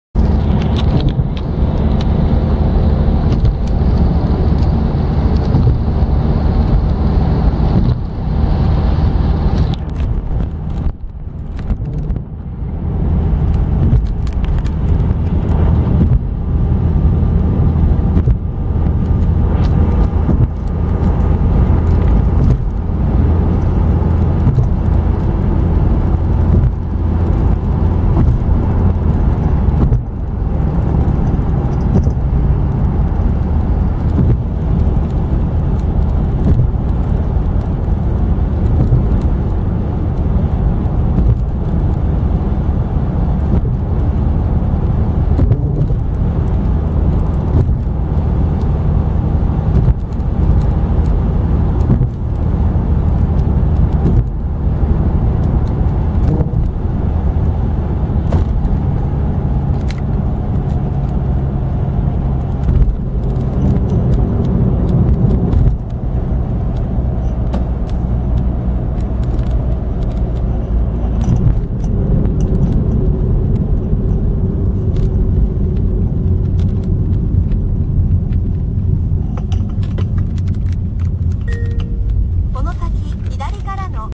R317 しまなみ海道 因島大橋 大浜PA 2016/01/01
台ダムを目指して、しまなみ海道を走行しています。